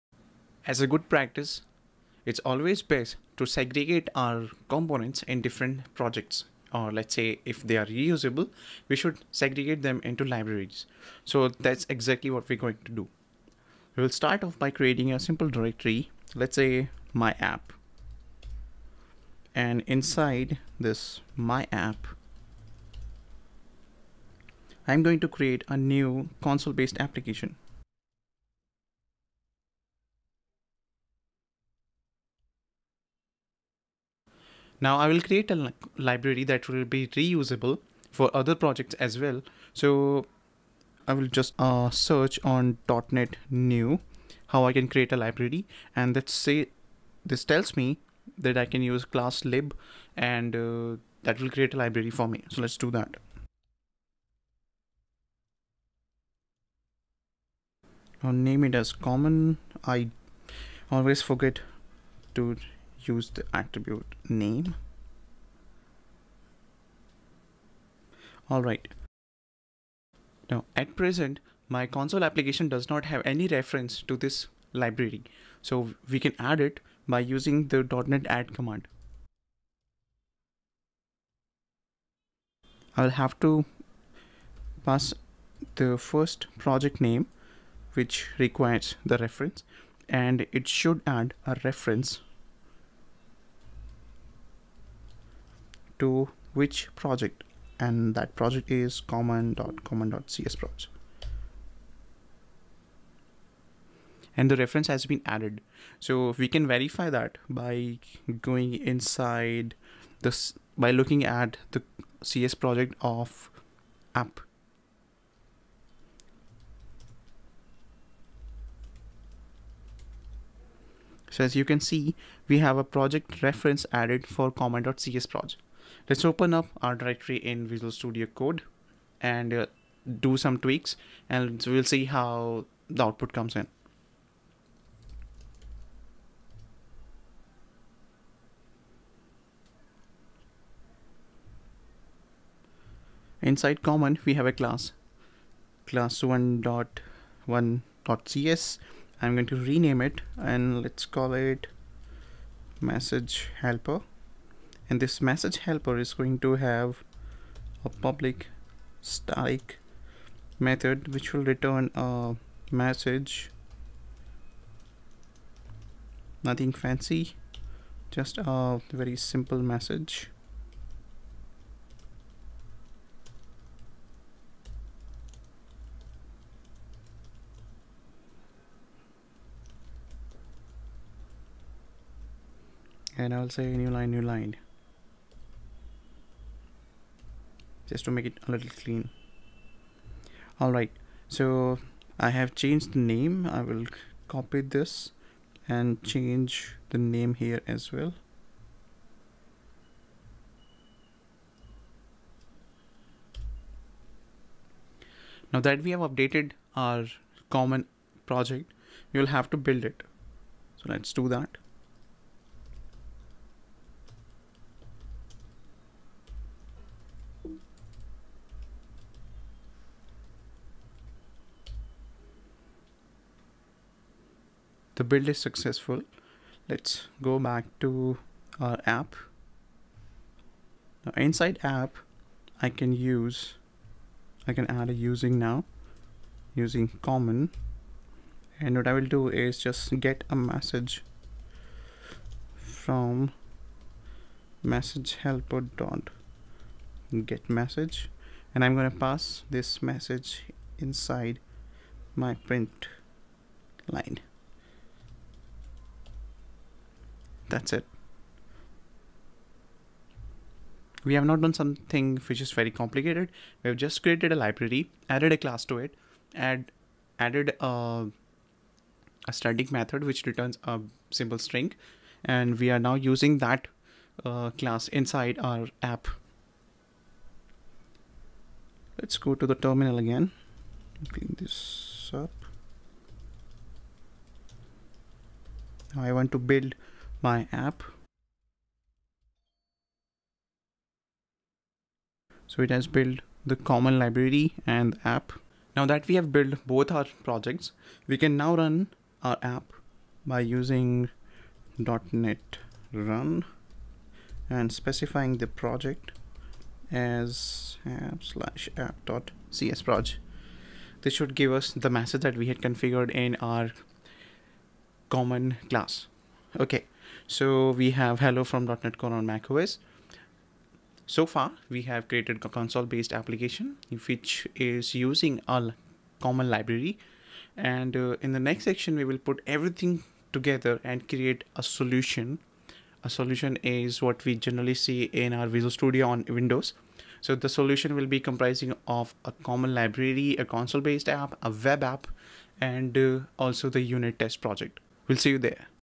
Adding a Class Library to your Project
Hello, World! :)

Welcome to the class, Getting Started with ASP .Net Core 2.1 on Mac OS. This class is basically the first one in a series of upcoming classes. In this class, we will setup our .Net Core 2.1 environment and will then create some projects to get familiar with .Net Core CLI (Command Line Interface).

During the class:

- we will first install .Net Core 2.1 on Mac OS.
see what we can do with the .Net Core CLI.
- create our very first project as a "Hello, World!" console app.
we will then create an MVC project.
- in real life, Web apps or any app for that matter, consume different projects or libraries, and therefore, we will add a class library to our app too.
- after that, we will create a Visual Studio Solution that will contain a console based app, an MVC project, and a class library that will be referenced by both the apps.
- we will then add an NUnit test project to our solution and add some tests.
- we will run those tests to make sure everything is fine.

And not to mention, all of this using the .Net Core CLI.